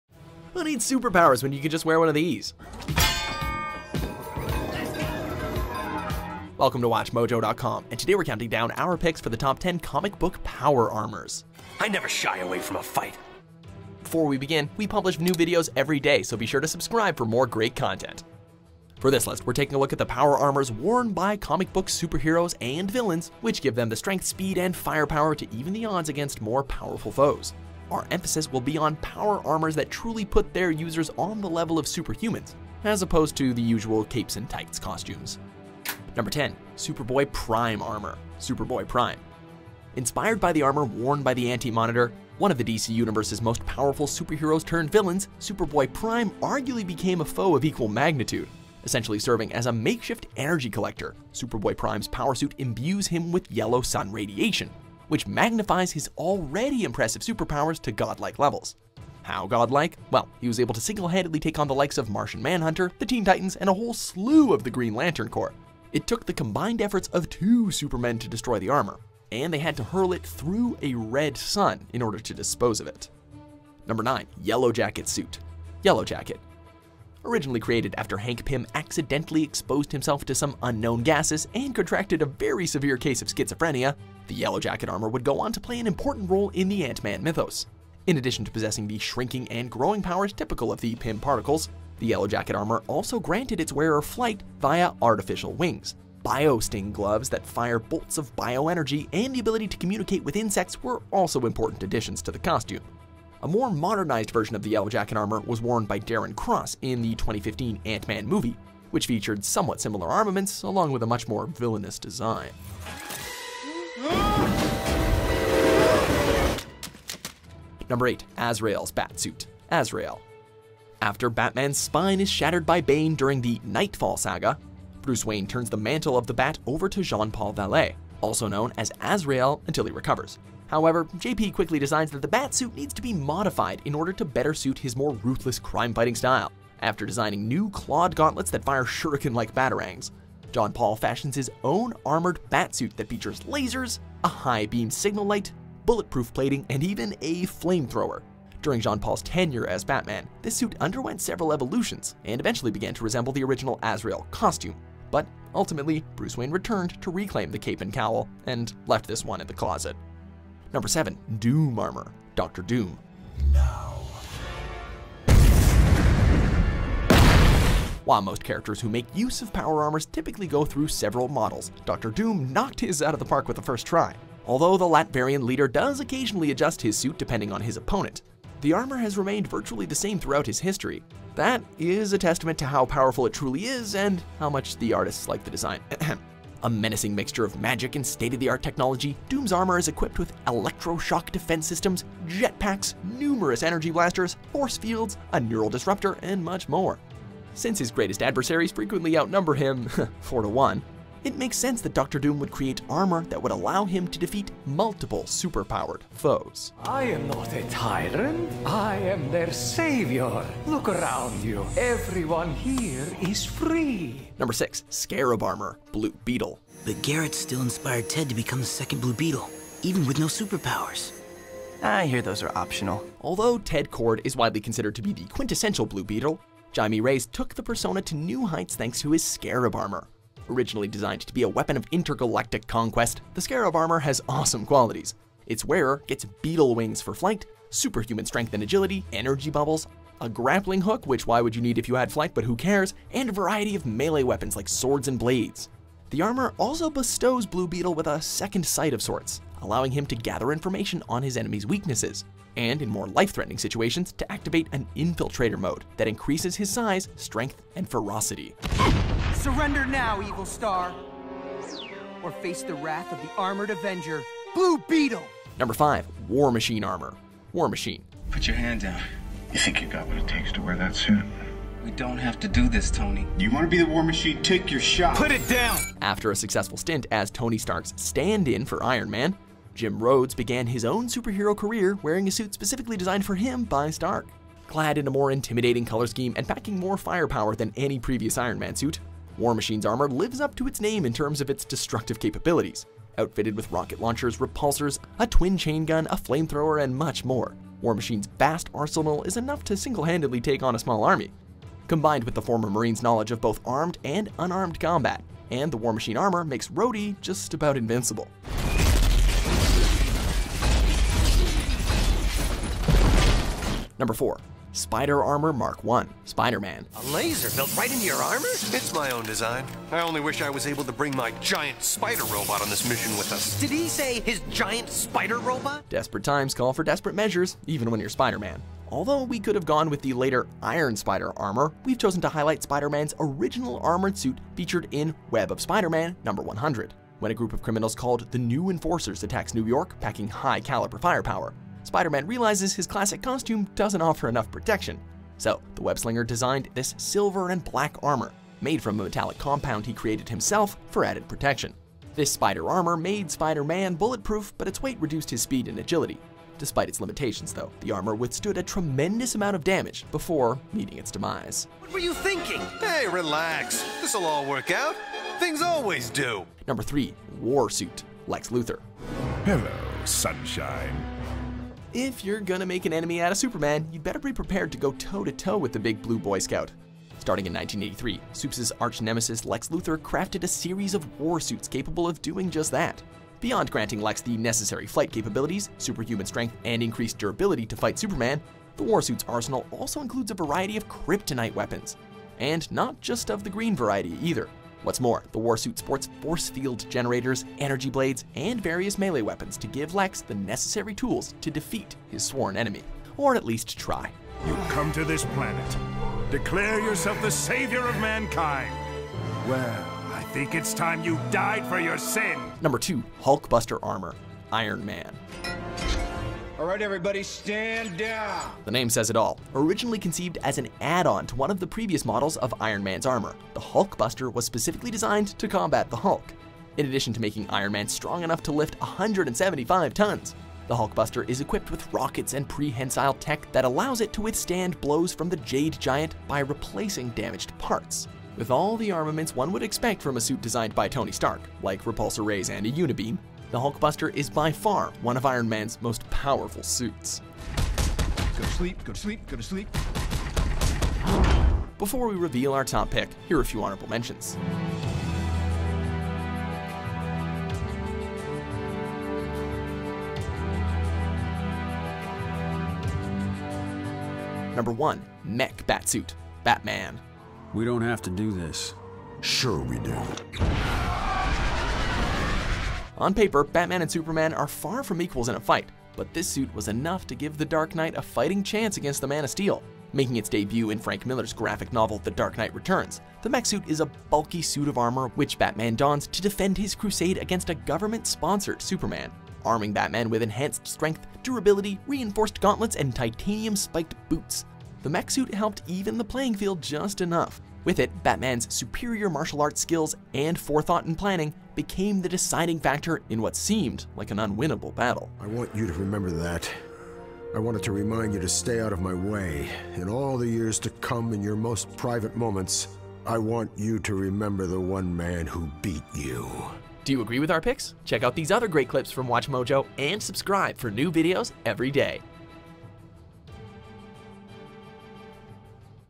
Top 10 Comic Book Power Armors
Be sure to visit our Suggest Tool and Submit Ideas that you would like to see made into Top 10 videos!

Okay, let's face it: in most cases, superheroes and power armor don't always go together, oftentimes leading to some preposterous design choices. That being said, these are the gold standard of comic book power armors, and the exception to the laundry list of poorly done power armors throughout comic book history.

00:36 #10: Superboy-Prime Armor - Superboy-Prime
01:13 #9: Yellowjacket Suit - Yellowjacket
01:57 #8: Azrael’s Batsuit - Azrael
02:41 #7: Doom Armor - Doctor Doom
03:33 #6: Scarab Armor - Blue Beetle
04:25 #5: War Machine Armor - War Machine
05:13 #4: Spider-Armor MK 1 - Spider-Man
06:04 #3, #2 & #1: ???